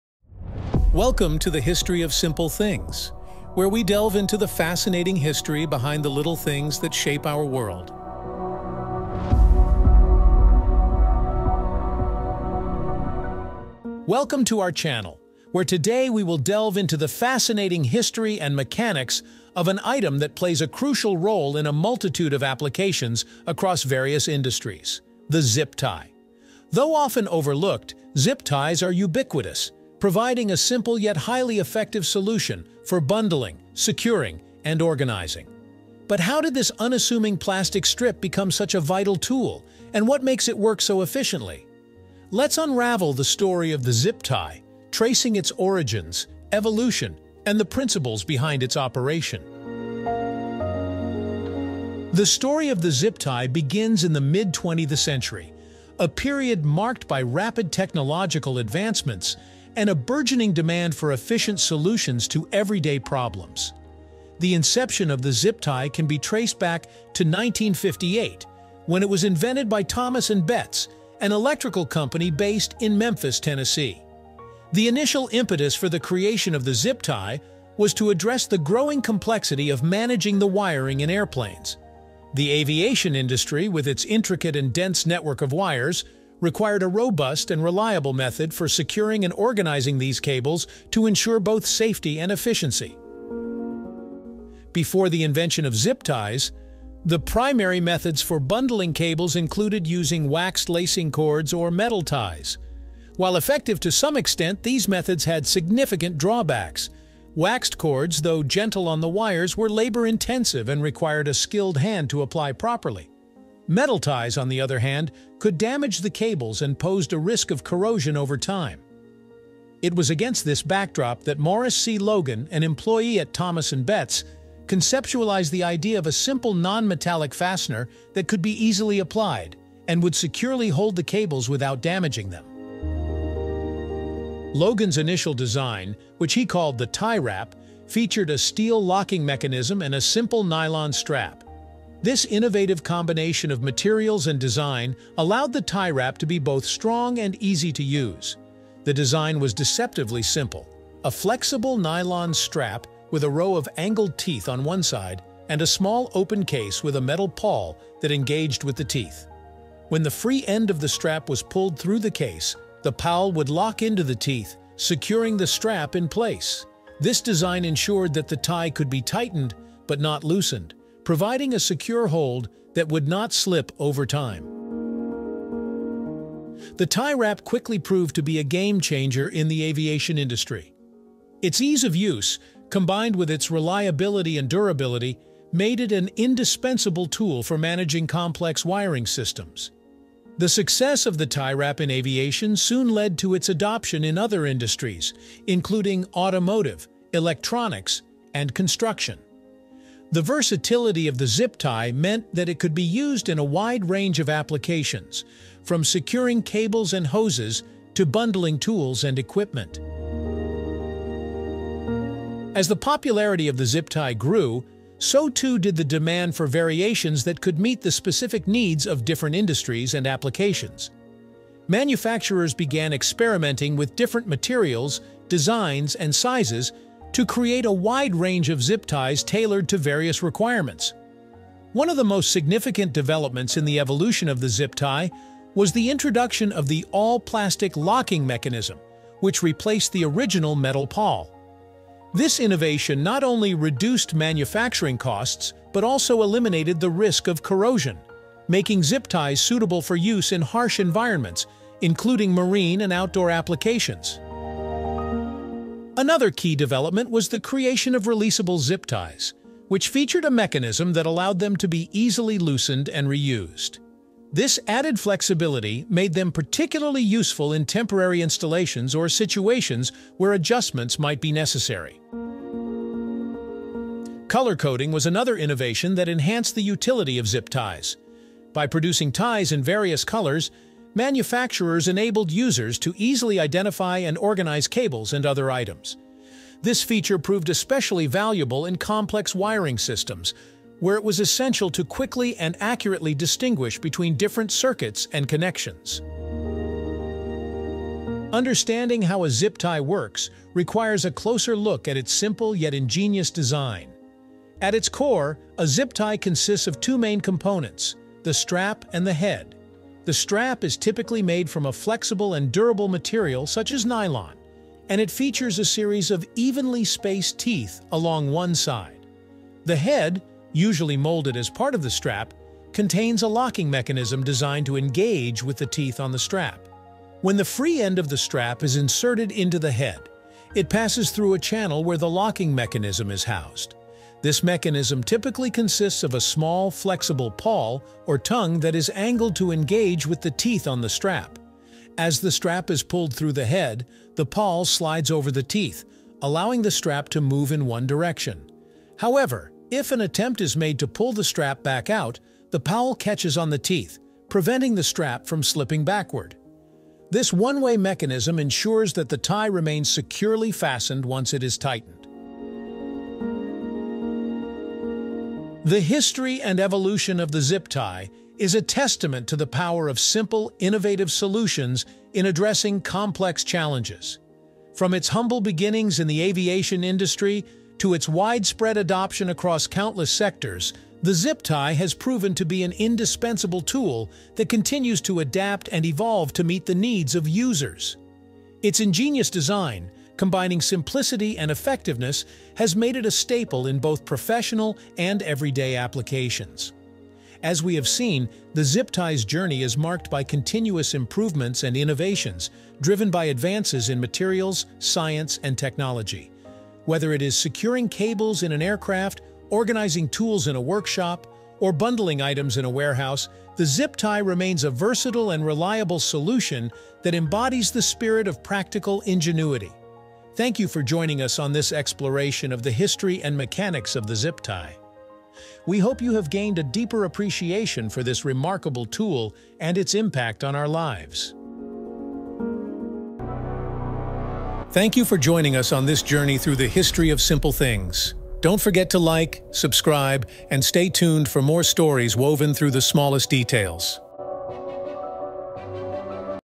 Who Invented The Zip Tie?
In this video, we delve into the fascinating history and mechanics of an essential yet often overlooked tool: the zip tie. From its invention in the late 1950s to its widespread use across various industries today, the zip tie has become indispensable for bundling, securing, and organizing. Join us as we explore the origins of the zip tie, its evolution, the ingenious design that makes it so effective, and its numerous applications.

Share your thoughts and experiences with zip ties in the comments below – we’d love to hear from you!